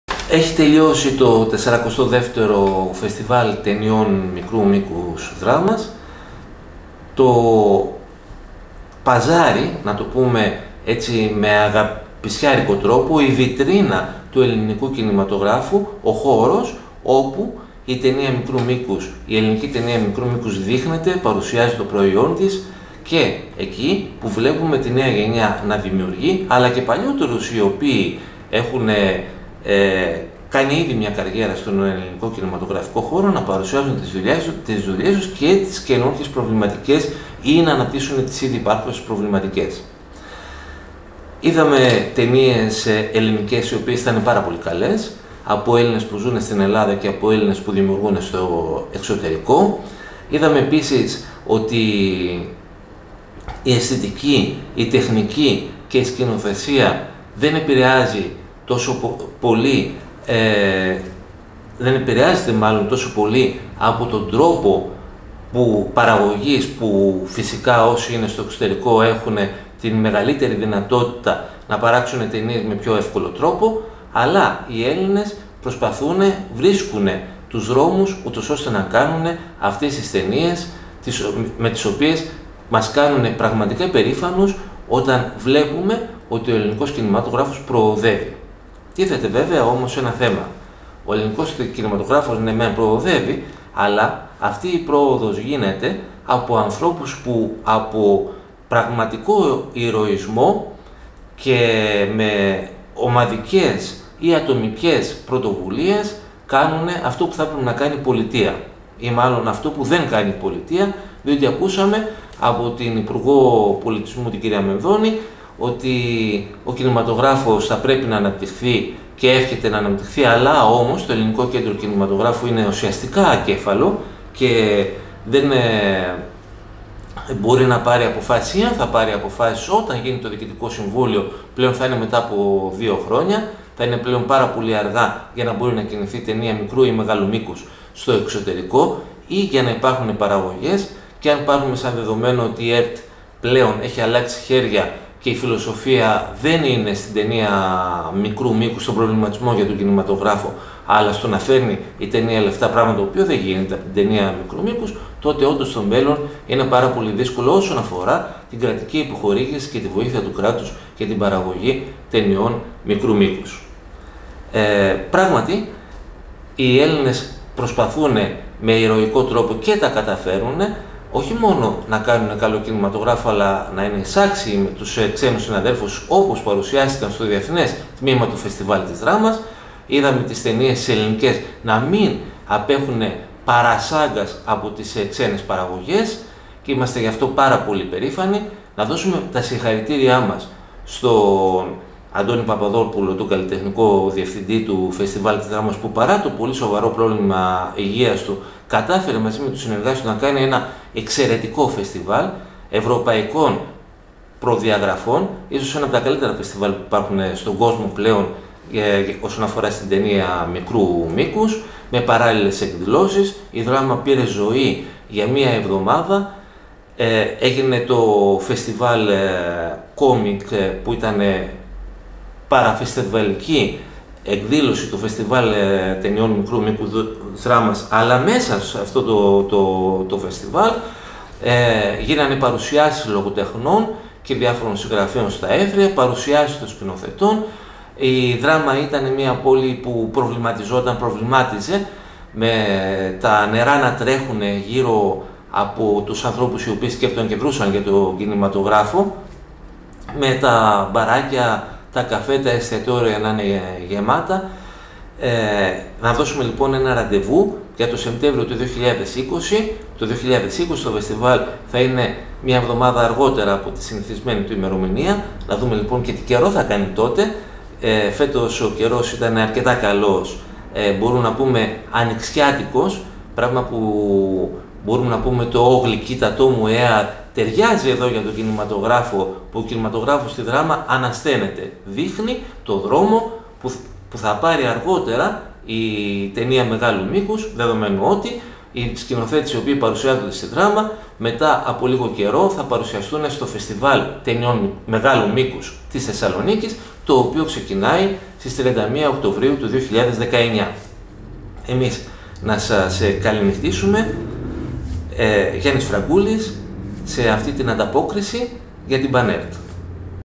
42ο ΦΕΣΤΙΒΑΛ ΤΑΙΝΙΩΝ ΜΙΚΡΟΥ ΜΗΚΟΥΣ ΔΡΑΜΑΣ
Μία γενική άποψη για το Φεστιβάλ Ταινιών Μικρού Μήκους Δράμας. Κάμερα-σκηνοθεσία: Γιάννης Φραγκούλης (κριτικός κινηματογράφου-παραστασιολόγος). Μία παραγωγή για την ΠΑΝ.Ε.Ρ.Τ.